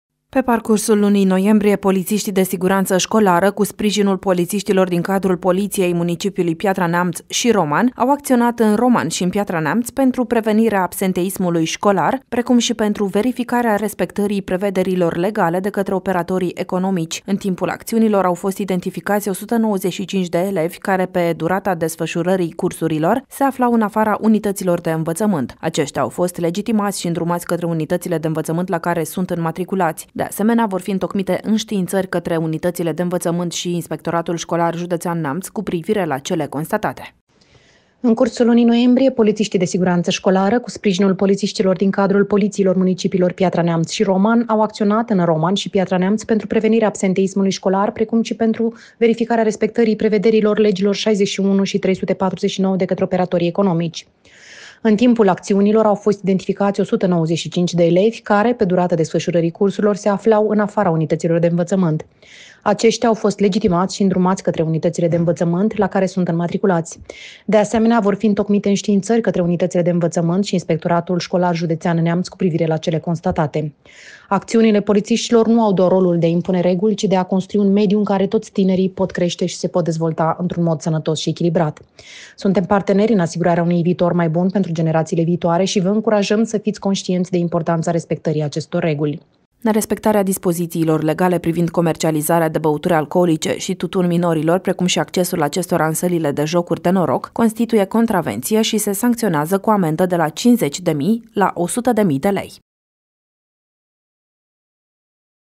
Polițiștii, pe urmele elevilor "chiulangii"
În cursul lunii noiembrie, polițiștii de siguranță școlară cu sprijinul polițiștilor din cadrul Poliției municipiului Piatra Neamț și Roman, au acționat în Roman și în Piatra Neamț pentru prevenirea absenteismului școlar, precum și pentru verificarea respectării prevederilor legale de către operatorii economici. În timpul acțiunilor, au fost identificați 195 de elevi care, pe durata desfășurării cursurilor, se aflau în afara unităților de învățământ. Aceștia au fost legitimați și îndrumați către unitățile de învățământ la care sunt înmatriculați. De asemenea, vor fi întocmite înștiințări către unitățile de învățământ și ISJ Neamț cu privire la cele constatate.